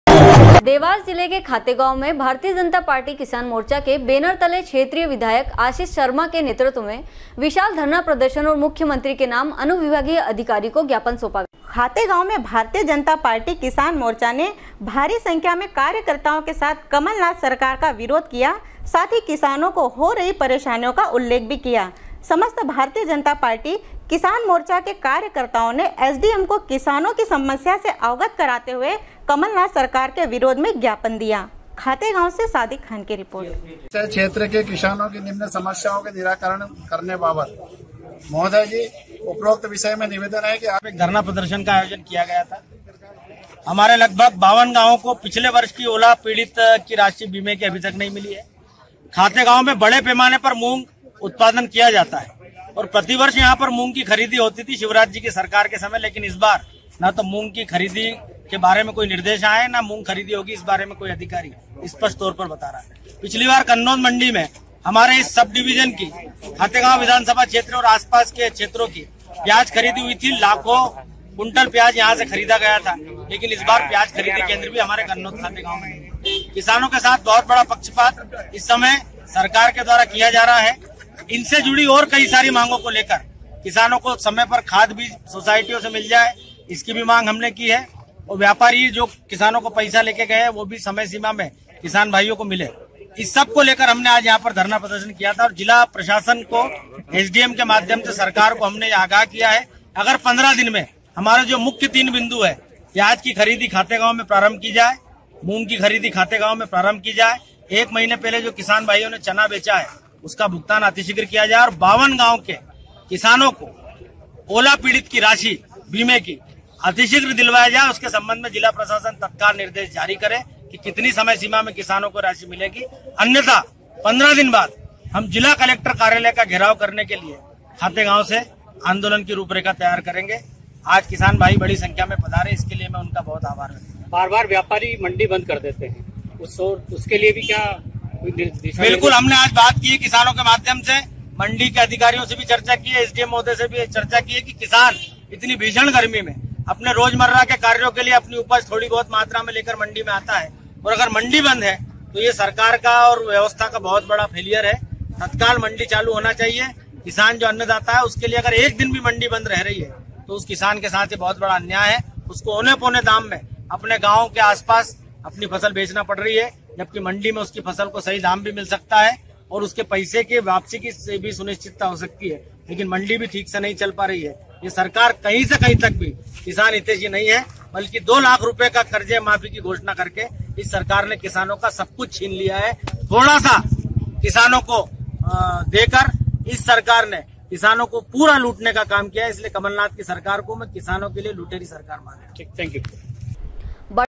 sw24news khategoan, 10.06.2019
देवास जिले के खातेगांव में भारतीय जनता पार्टी किसान मोर्चा के बेनर तले क्षेत्रीय विधायक आशीष शर्मा के नेतृत्व में विशाल धरना प्रदर्शन और मुख्यमंत्री के नाम अनुविभागीय अधिकारी को ज्ञापन सौंपा गया